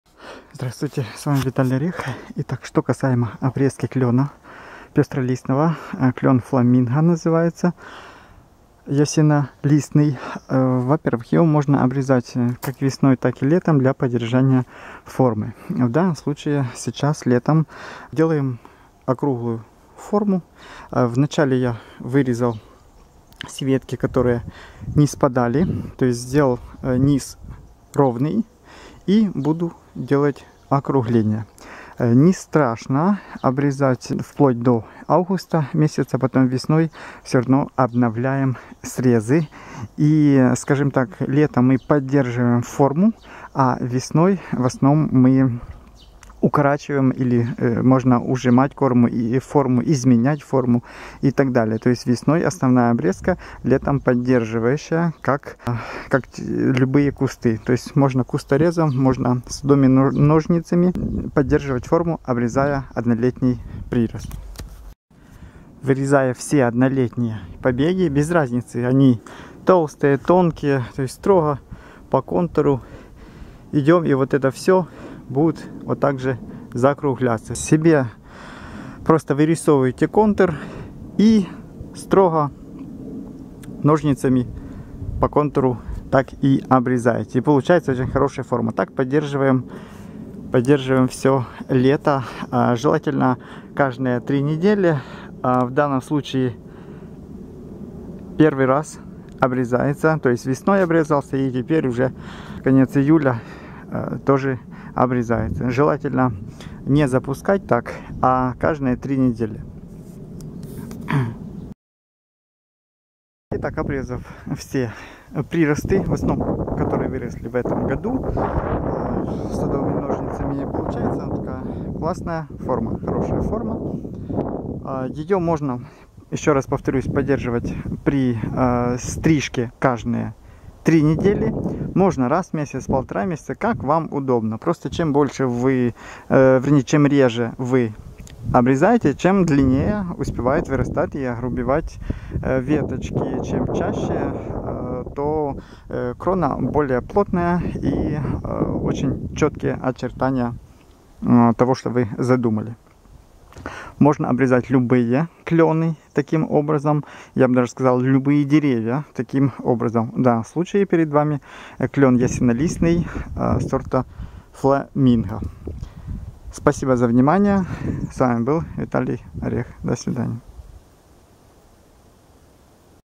Фигурная стрижка клена Фламинго.
Фигурная стрижка клена Фламинго. Начинаем обрезку с весны, делаем "скелет" формы,  далее каждые 3 недели применяем стрижку. Чем чаще стрижем, тем плотнее форма и ровнее будет.